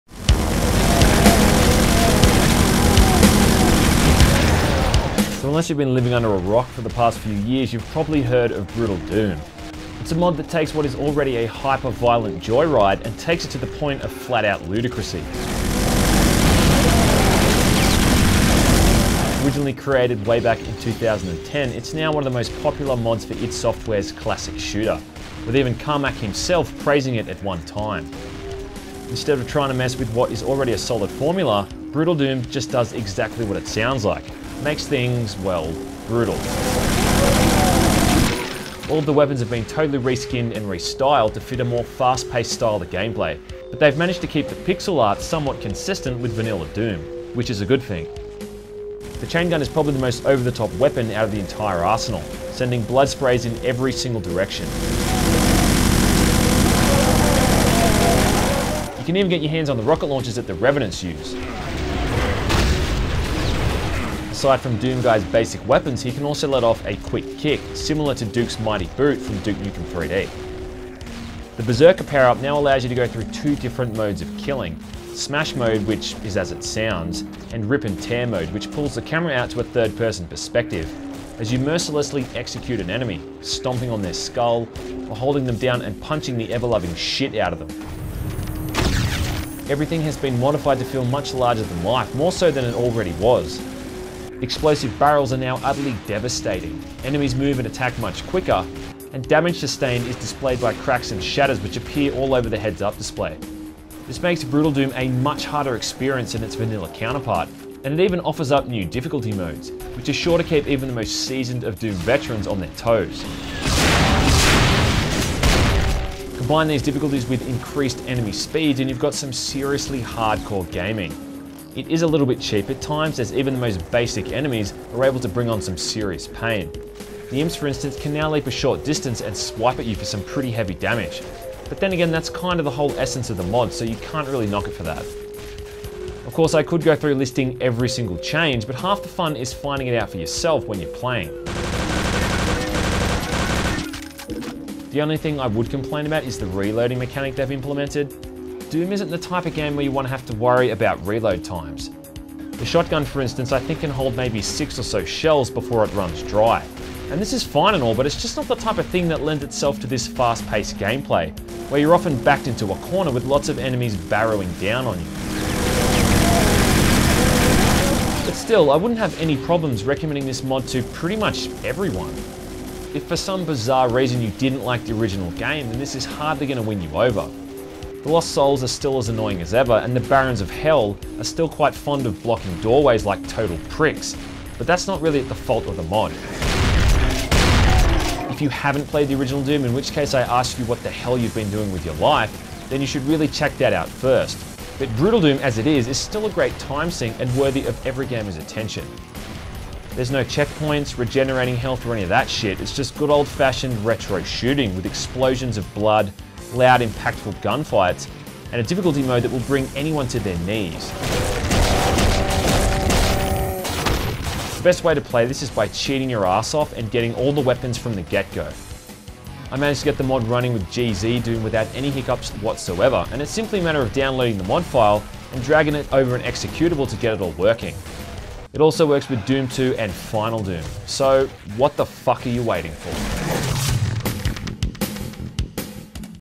Mod Corner - Brutal Doom Review
In development since 2010, the 18th Cacoward winner and Moddb 2012 MOTY Editors Choice of Creativity Award  Brütal Doom takes Doom into a whole new level. It makes the game much more violent than before. There's much more blood, plus it adds unique gibs, death animations, dismemberments, headshots, executions, fire and explosion particles, flares, shadows over all objects, and much more.

The whole world becomes more real and interactive.You can kick severed limbs and heads (and sometimes, use it to activate some traps in the map earlier, or even kick them against enemies to do some damage), you can shoot enemies' heads to deal more damage (and cause more gruesome deaths) you can destroy most things in the the scenario (trees, lamps, hanging bodies, etc), you can paint the floor, the walls, and even the ceiling with blood, you can push the explosive barrels to make traps, or grab them and use like an explosive weapon, you can silently take down enemies from behind and perform stealth kills, the chainsaw actually cuts the enemies in two. When you find the berserk pack and get super strength, you can perform cinematic executions and RIP AND TEAR your foes with your bare hands.

Some enemies will scream in anguish and try to crawl away when near death, and they can be used as human shields, and much, much more

Everything in Brutal Doom is extremely intense. Everything sounds louder, looks bigger, moves faster, and hits harder. You can even fell the impact of explosions. Enemies are harder and smarter, and weapons and explosions are loud as fuck.

Your weapons have been upgraded to a deadly arsenal, completely throwing the vanilla Doom's "progressive weapon system" through the window. Every weapon is powerful and have it's pros and cons, and a weapon will never be outclassed when you find a new weapon.You have new abilities too. With any weapon selected, you can press Q (default bind) to kick enemies at close range and send them away. Press F (default bind) to taunt enemy players, and Z or X (default binds) to wave to other players in cooperative modes.

Brütal Doom will make the good an old Doom have a new and fresh look. It will give you a reason to play all the Ultimate Doom, Doom 2, and Final Doom levels again. This mod also works with almost every megawad and map pack around. Take a breath, and prepare to RIP AND TEAR HUGE GUTS!